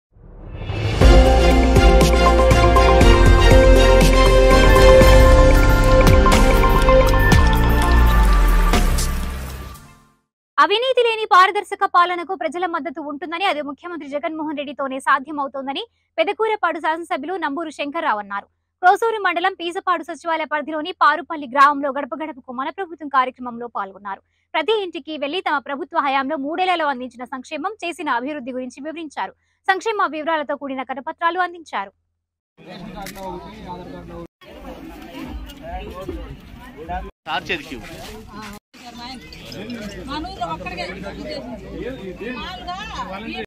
అవినీతిలేని పారదర్శక పాలనకు ప్రజల మద్దతు ఉంటుందని.. అది ముఖ్యమంత్రి జగన్ మోహన్ రెడ్డి తోనే
అవినీతిలేని పారదర్శక పాలనకు ప్రజల మద్దతు ఉంటుందని.. అది ముఖ్యమంత్రి జగన్ మోహన్ రెడ్డి తోనే సాధ్యమవుతోందని